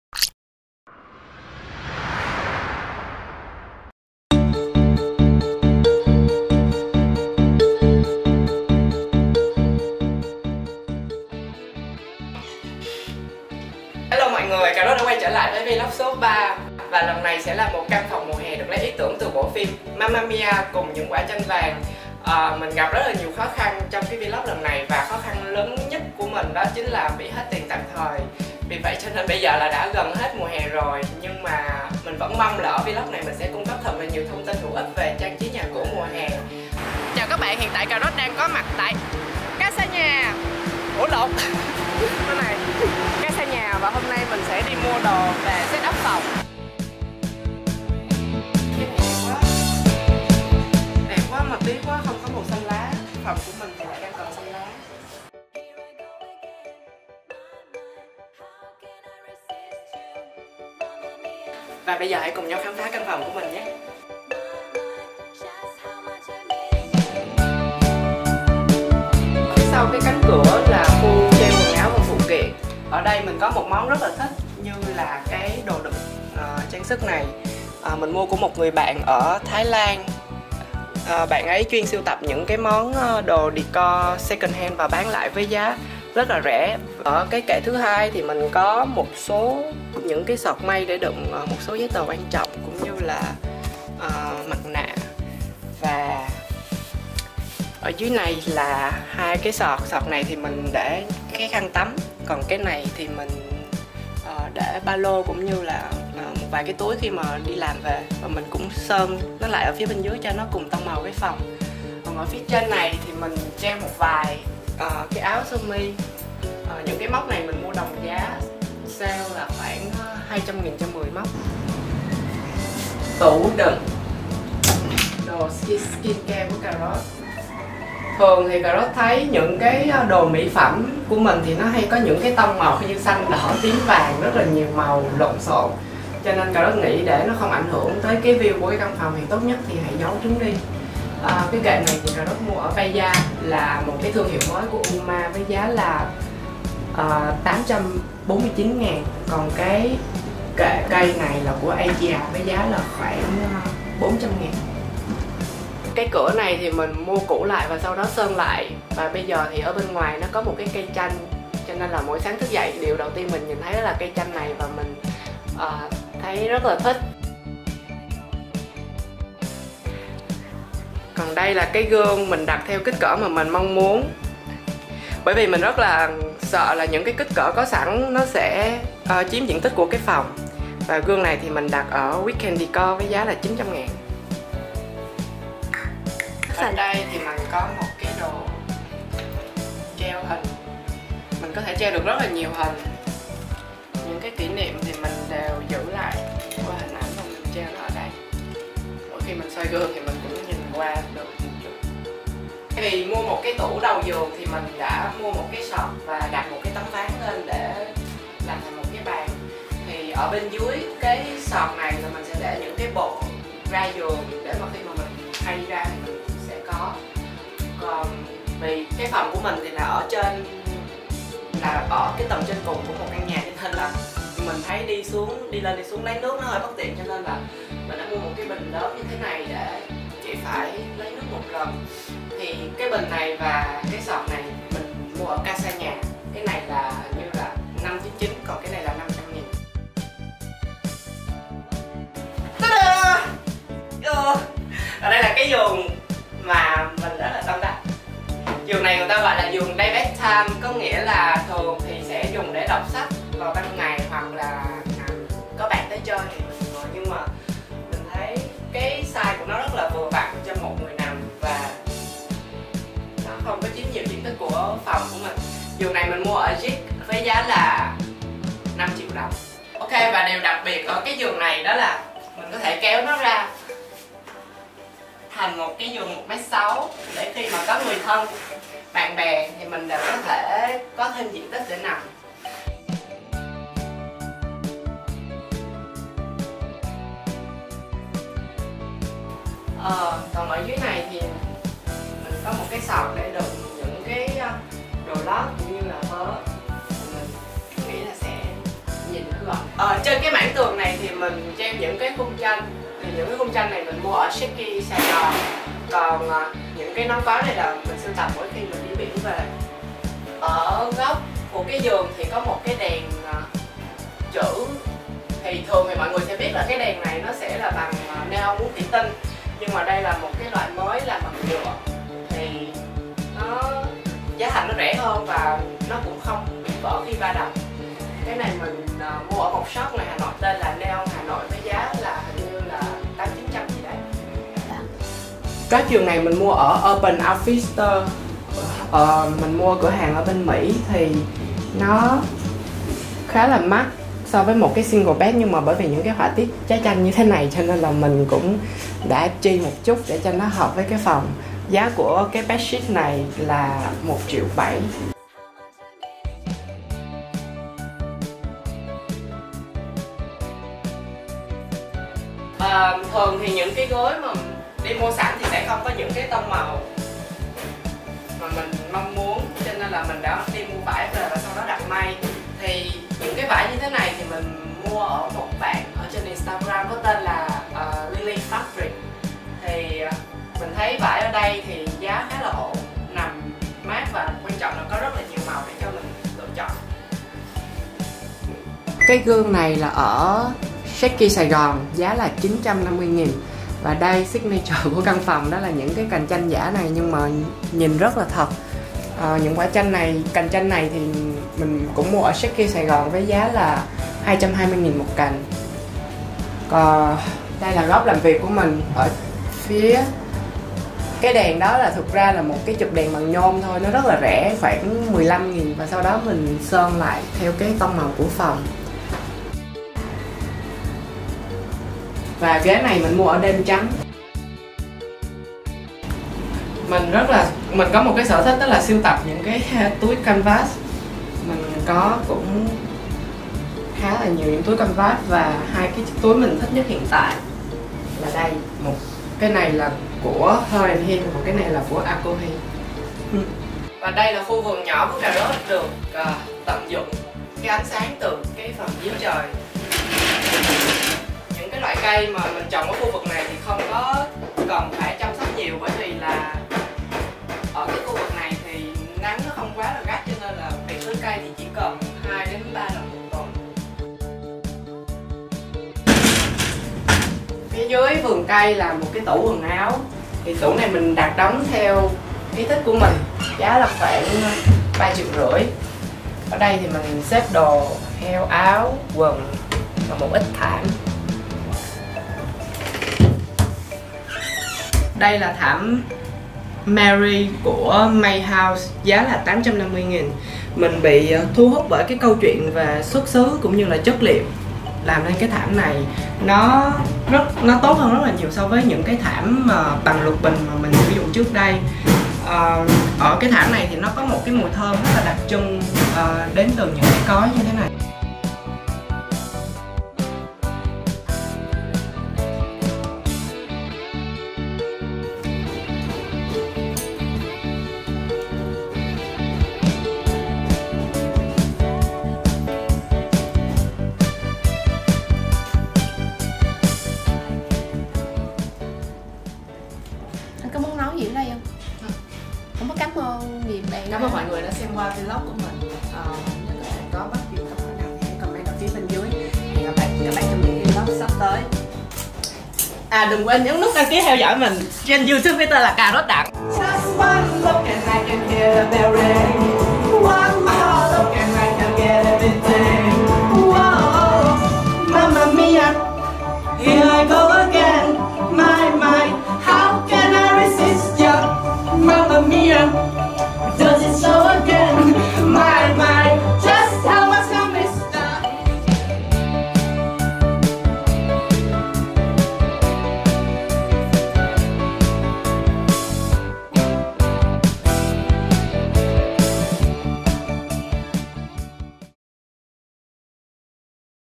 Lemon Summer Room 2019 🍋 | CARROT DANG | ROOM TOUR
Hi everyone,
🍋I'll take you back to my room on summer themed . Let's enjoy it!